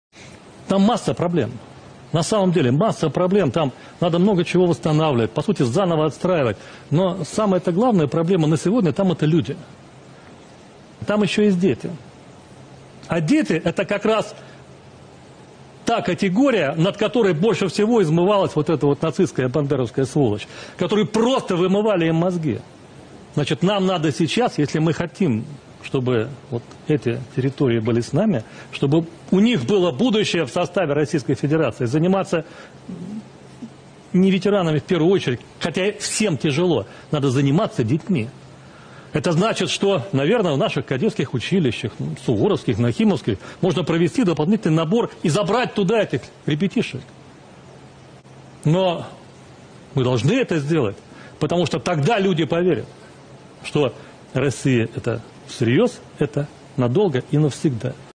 А. Картаполов предложил забирать украинских детей и отправлять их в российские военные училища.🤦‍♂️
Госдумы по Обороне Андрей Картаполов предложил забирать украинских детей с оккупированных территорий

и отправлять их в российские военные училища. Ни слова о том, чтобы спросить, для начала, родителей или хотя бы учесть пожелания самих детей.